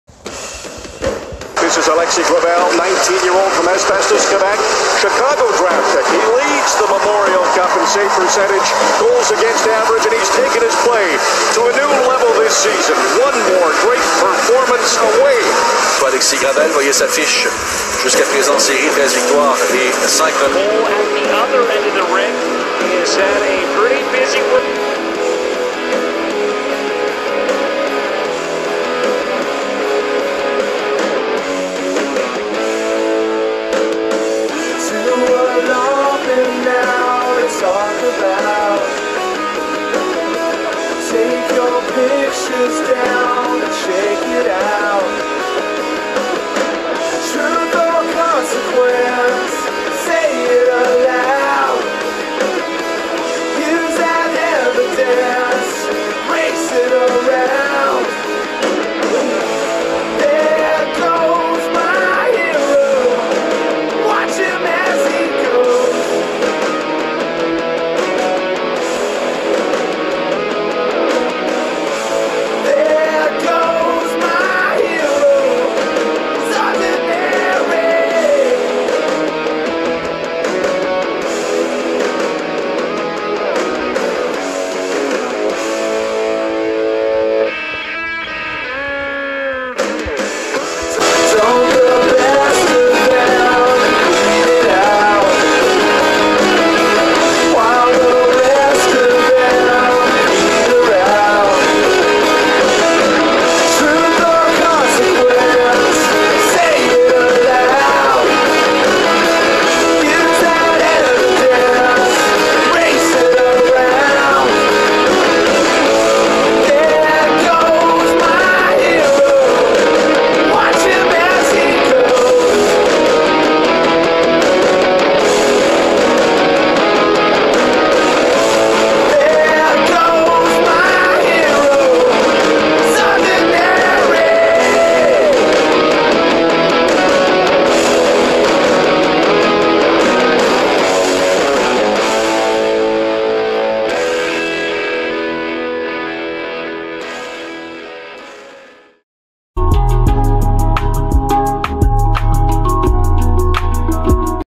AELEXIS GRAVEL QMJHL CAREER HIGHLIGHTS!
Thanks Alexis Gravel, I only got the chance to watch you on T.V. and on a screen, finally got the chance to watch you in person this season. when times were tough the Q has always been there to make my day, you were my favorite player and favorite goalie and always will be, good luck to where ever you play in the future, i know you will not read this but i do not thank you on behalf of the halifax mooseheads and how much you have done for them, im thanking you for how much you have done for me. and i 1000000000% mean that. thank you so much. i was just a kid from a small town in newfoundland with no hockey to watch rather then play street hockey my self. and the CHL and QMJHL was always my favorite thing even though i watch a game in person once every couple years except this season. thank you so much bro...

🇩🇪 1 🇨🇦

THANK YOU ALEX!